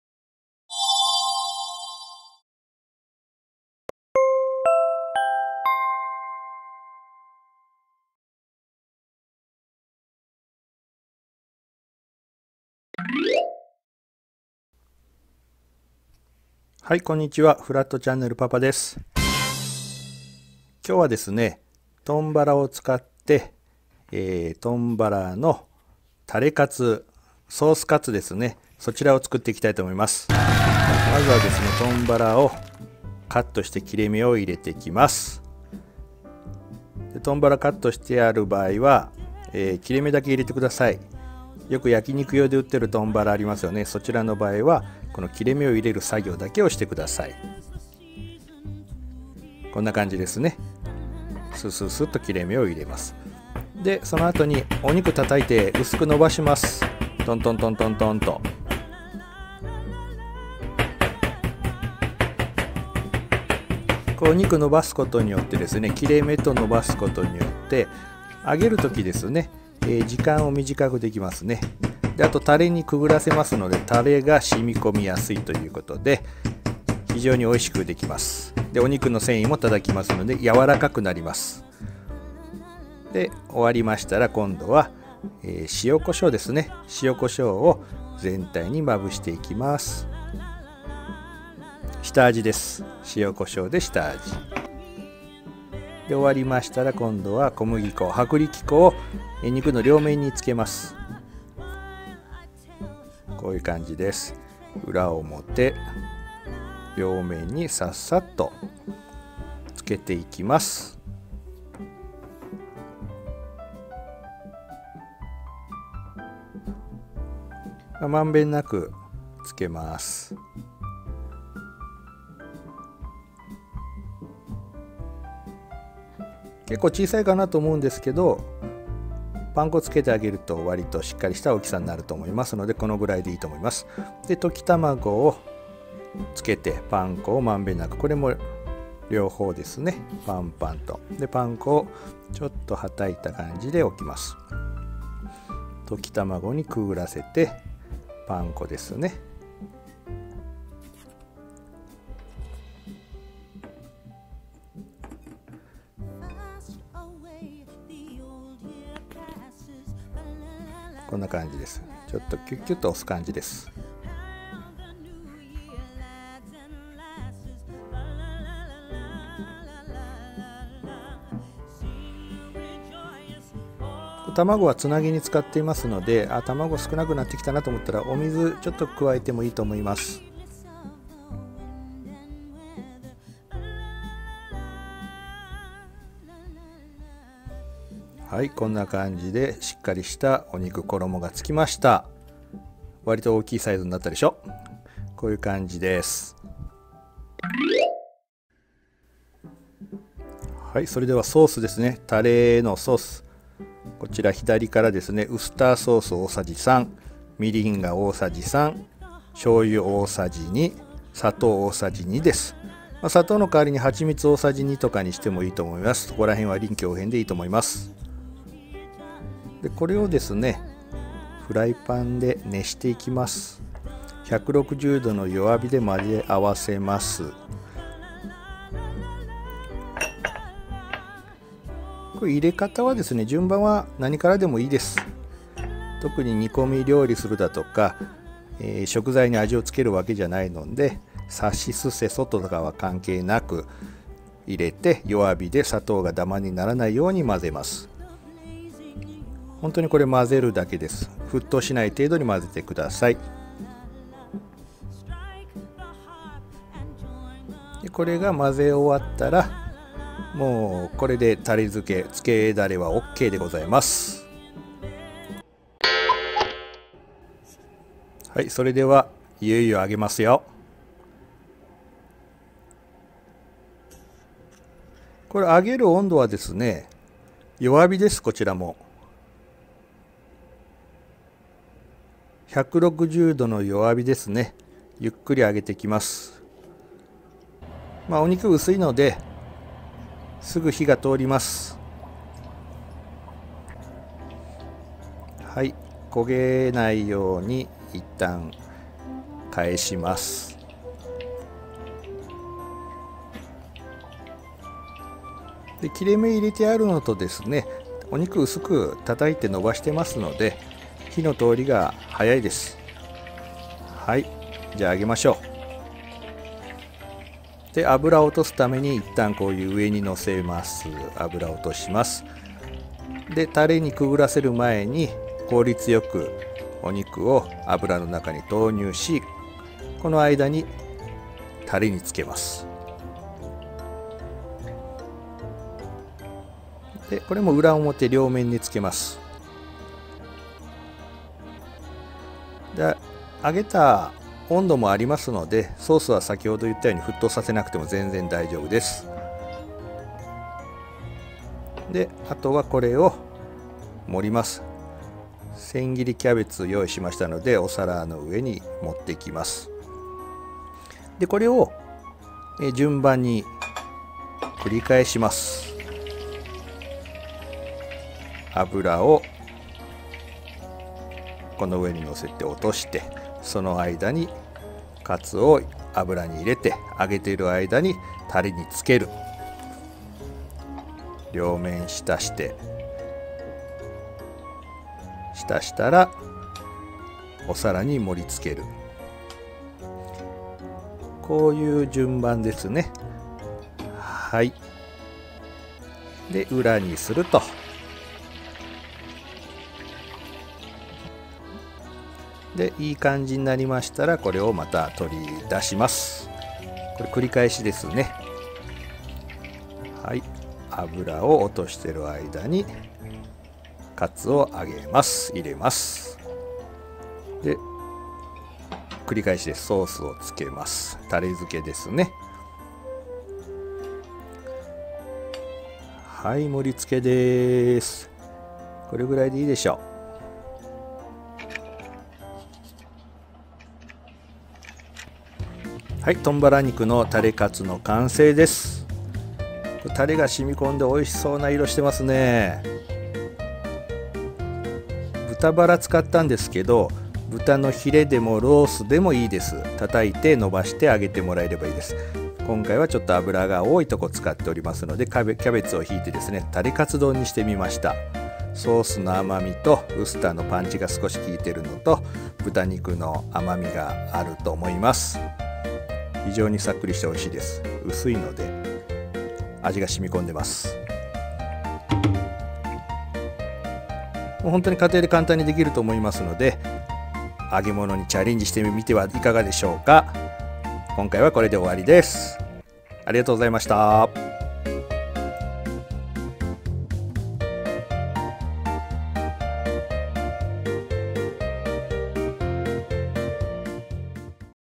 コストコの大量豚バラを消費するアイデア！家庭で簡単ソースカツ丼 タレも簡単に作れて旨い【ふらっとちゃんねるパパママレオくん】
食育で息子を東大へ導いたパパのアイデア料理レシピ🎵

コストコの量が多い豚バラを消費するためにアレンジして揚げてみました！トンカツ
　ソースカツ(たれカツ)

ソースカツ丼　　　　　たれカツ丼

いつもはヒレやロースで揚げるトンカツを今日はコストコの豚バラで揚げよう♪

切れ目入れて叩いて薄く延ばしてアゲアゲ～♪

アブラでアゲアゲ～♪

気分もアゲアゲ～♪

タレは自宅にあるもので作るのだぁ＼(^o^)／

ウスターに砂糖にしょう油に・・・みりん！

これさえあれば完ぺきなタレが出来るぜぇｗｗｗ

ソースカツ丼　たれカツ丼　名前は地域により違うが

うまければ文句ないでしょう！！！w

てなわけで今夜は揚げ物いかがでしょうか？

参考になれば幸いです＼(^o^)／♪

ぜひ最後まで見てねぇ~

ふらっとちゃんねるパパママレオくん